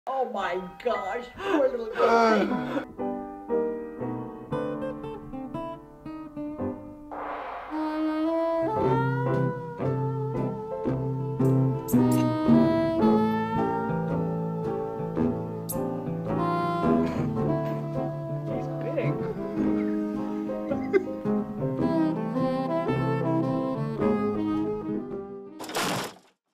Kangaroo Rat Stuck in Outlet Rescued | The Dodo
Kangaroo Rat Gets Rescued From Outlet | These people found a kangaroo rat trapped in an electric outlet and knew they had to help.